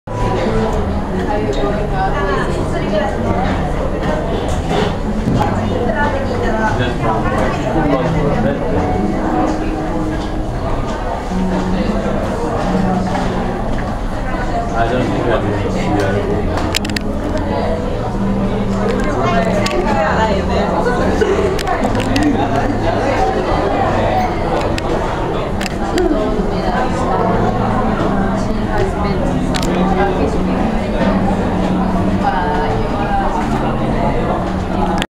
Shibuya Crossing
Spent a good hour sipping coffee watching the mass of humanity carefully coalesce into itself and then disassemble in ordered chaos.  Repeat every 80 seconds.